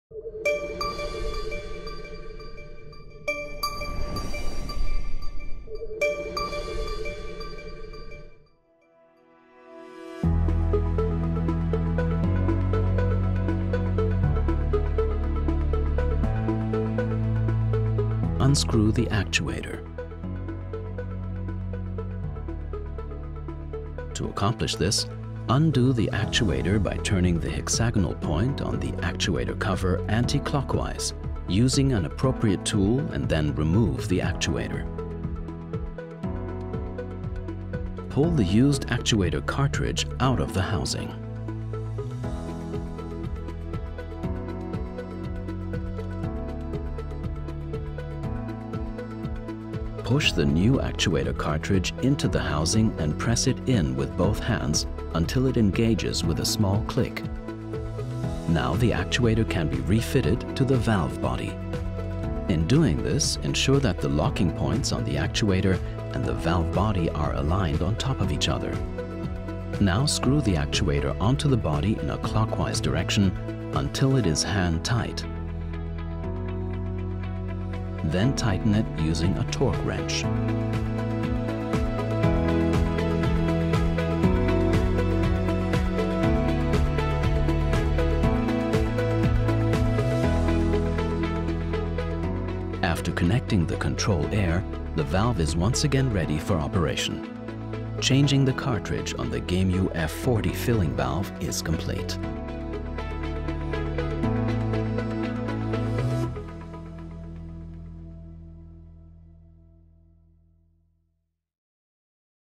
Changing cartridges of GEMÜ F40
Thanks to the Quick-Lock connection system, the GEMÜ F40 filling valve is quick and easy to service. It uses an innovative cartridge spare part system that enables all the necessary wearing parts to be replaced in a single step. The system also includes the plug diaphragm, which is affixed to the system but which can be ordered and replaced separately as a spare part as well. All that's needed to replace the cartridge and the diaphragm is a hexagonal torque wrench. Actuator size AG 3 must be tightened to at least 60 Nm. There is also no need to retighten the seal, which offers additional time savings. Further information and practical tips are provided by the GEMÜ training team in a number of training courses.